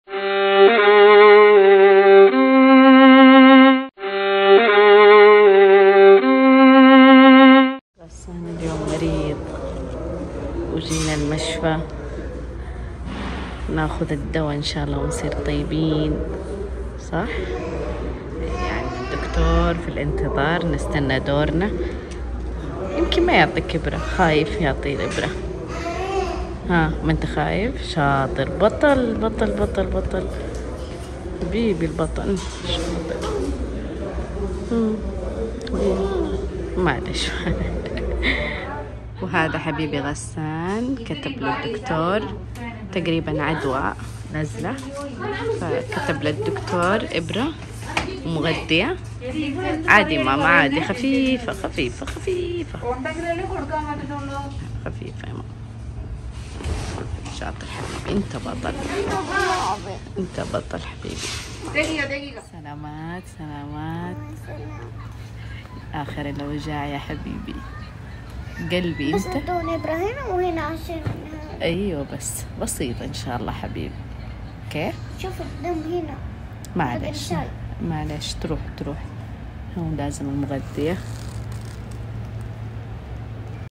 الدكتور يعالج غسان مريض || ابرة ومغذي 🥺💔
الدكتور يعالج غسان ويعطيه ابرة 🔥💔

نصور يومياتنا وطلعاتنا وايش يصير معانا من اكشن وتحديات ومقالب اللايك والاشتراك دعم لنا نحبكم❤️❤️

لاتنسون تسون لنا اشتراك بالقناه..